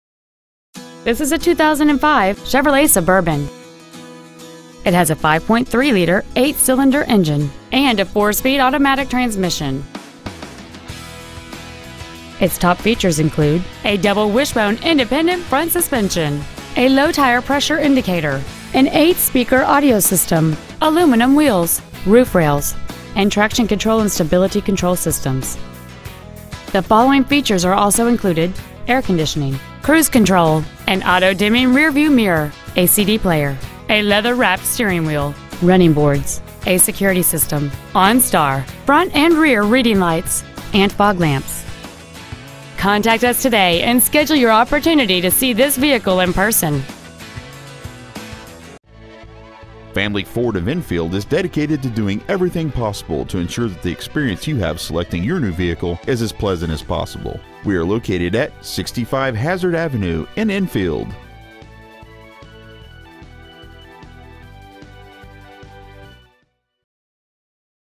Used 2005 CHEVROLET SUBURBAN Enfield CT
We have been honored to serve the Enfield CT area , we promise that your experience at our dealership will exceed your expectations !

 Year : 2005
 Make : CHEVROLET
 Model : SUBURBAN
 Engine : 5.3L V8 16V MPFI OHV FLEXIBLE FUEL
 Trans . : 4-SPEED AUTOMATIC
 Exterior : BLACK
 Miles : 127,807
 Interior : TAN / NEUTRAL
 Stock : N0516A